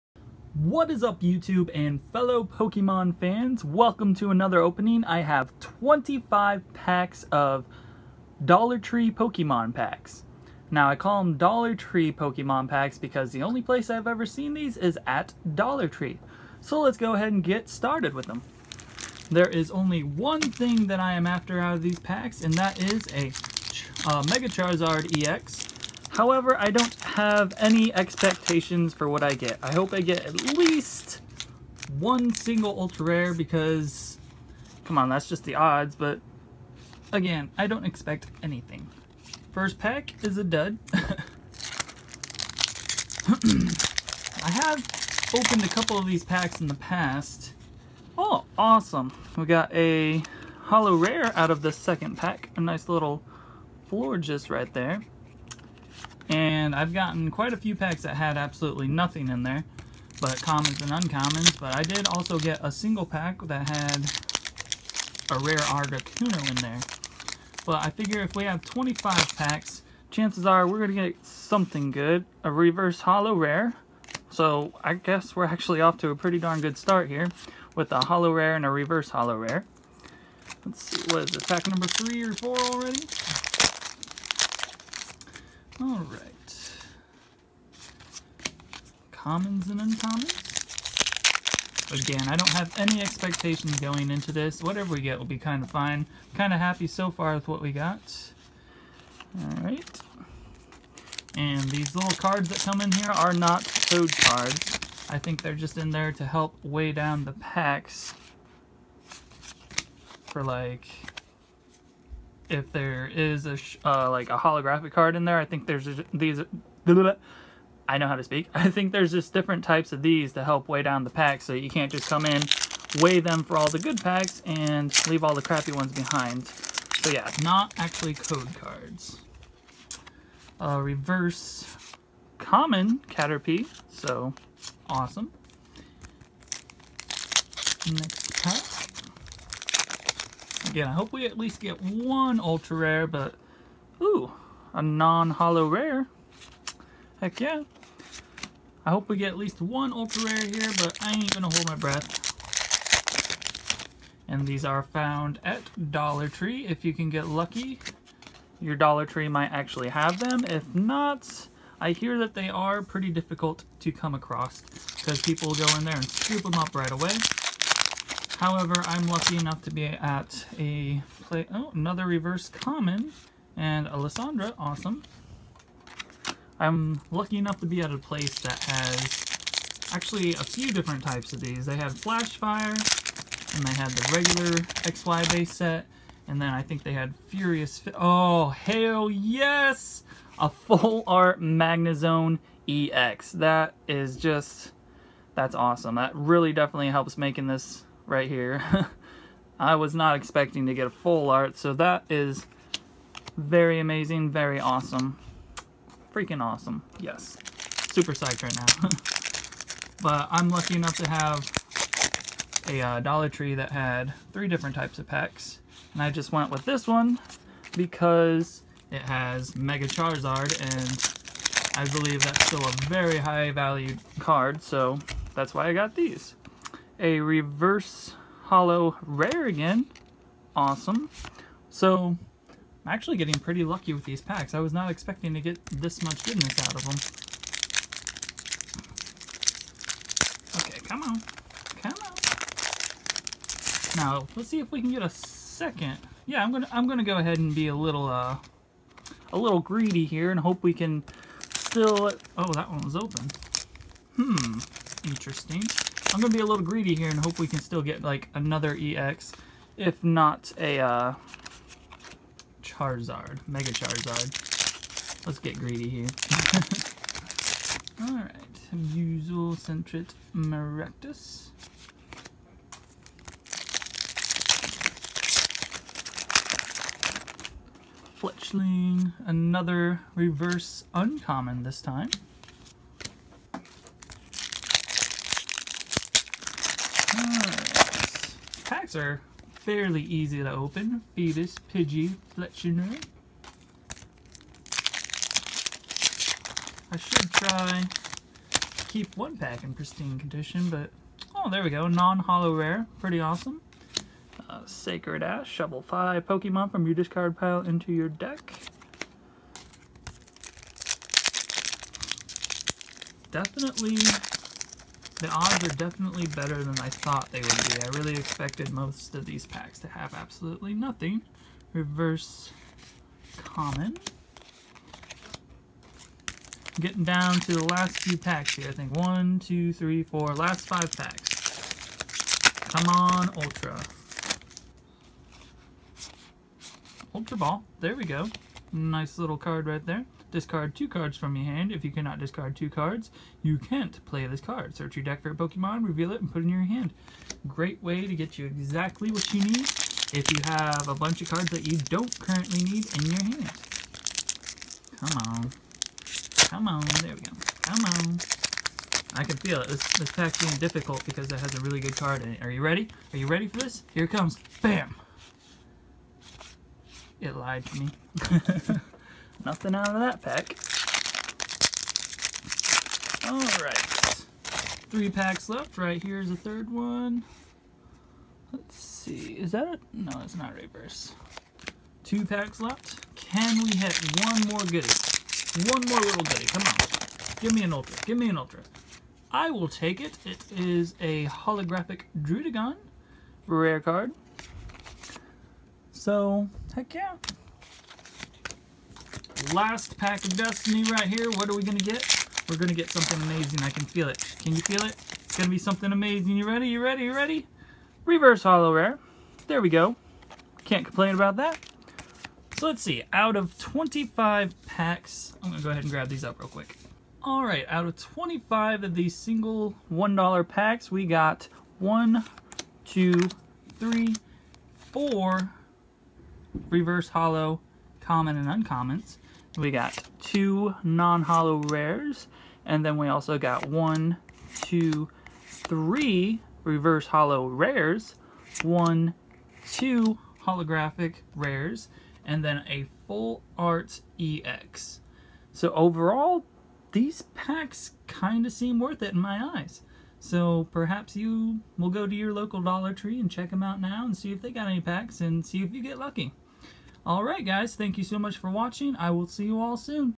Opening 25 Flashfire Dollar Tree Pokemon Packs
That's right, Dollar Tree has pokemon cards for a dollar, and I got 25 of them, can we pull some ultras?